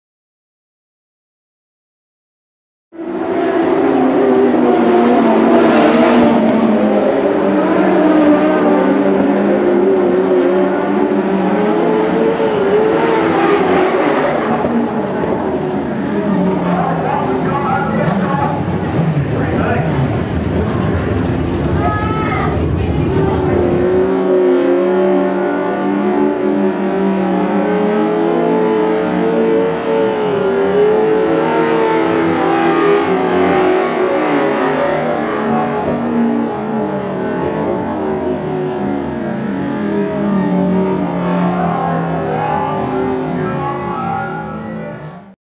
Lightning Sprints: Scott Genrich Rollover - Charlton Raceway
Scott Genrich makes wheel to wheel contact and rolls on the back straight at Charlton Raceway on the 17.12.11